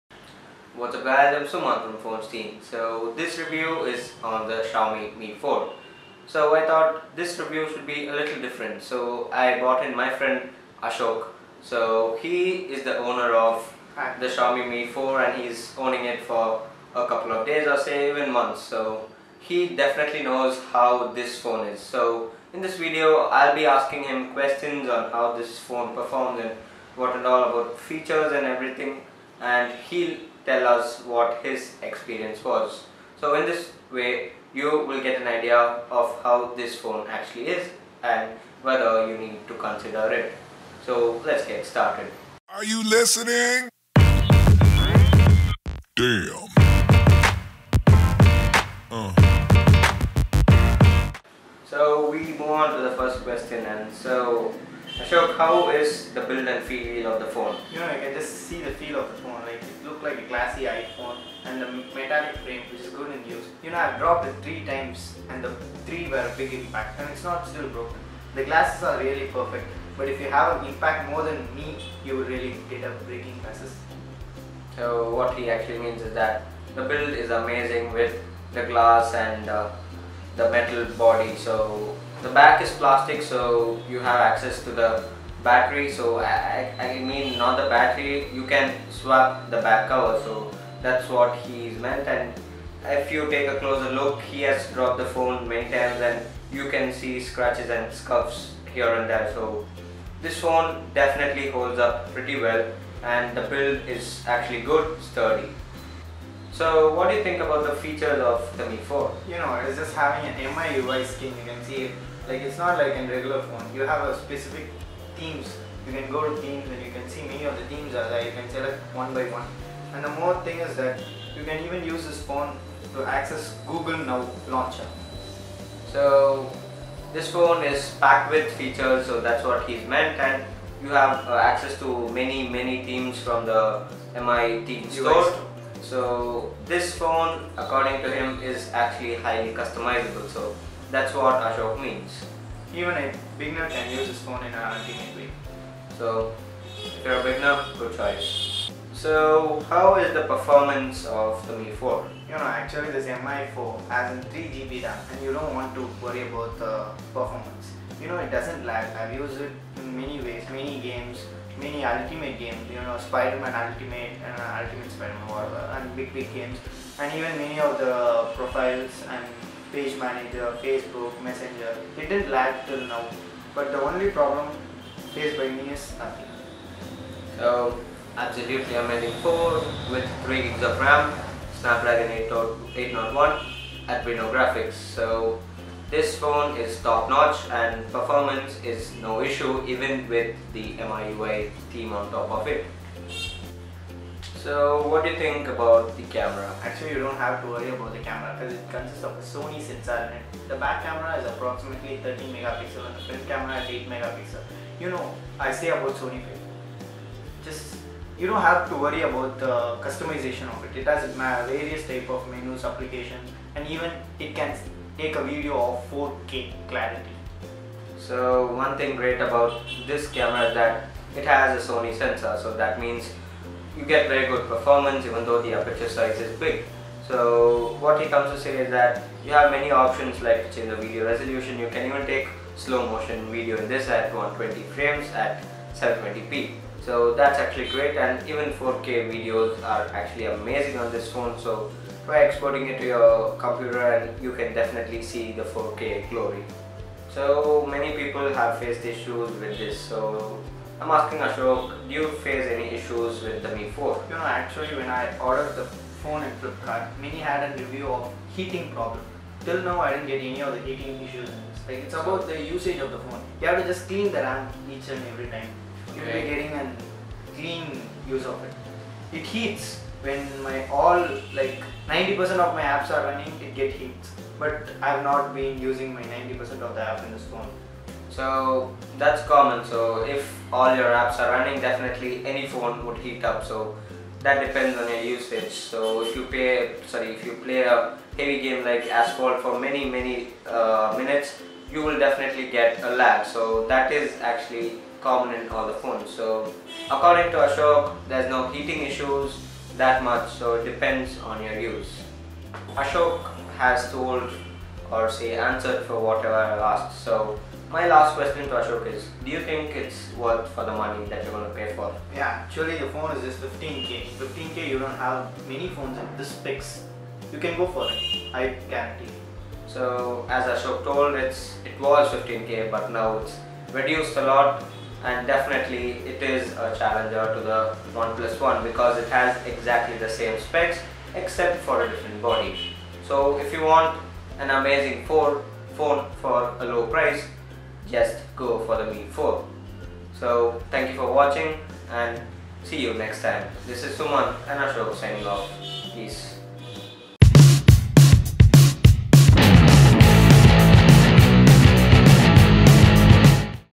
Xiaomi Mi4 In-depth Review (After 2 Months) !
The Killer Mobile They say !! But is it actually Killer ?! Let's find out .